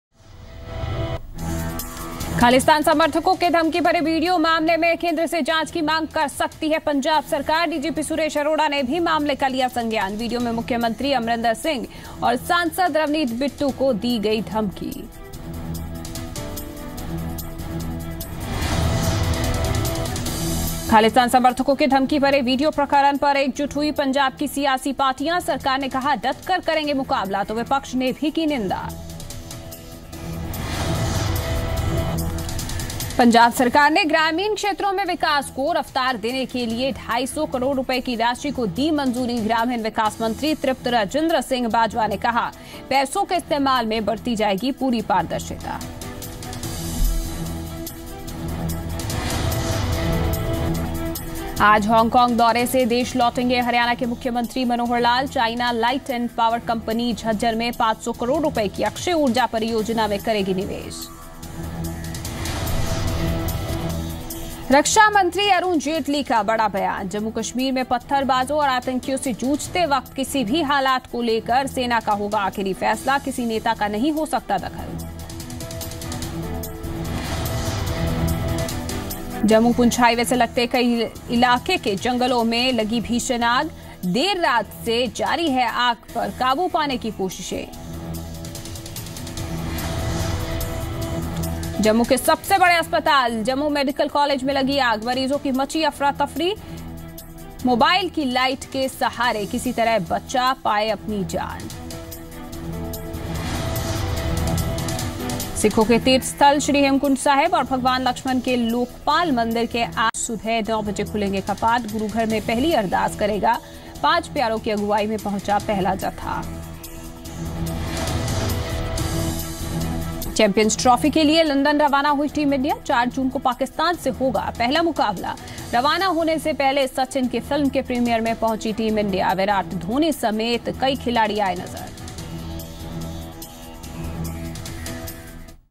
Punjab govt may approach central govt to investigate Khalistan threat video
Punjab govt may approach central govt to investigate Khalistan threat video. Watch complete news story of Headlines @ 9:00 AM for getting the detailed news updates!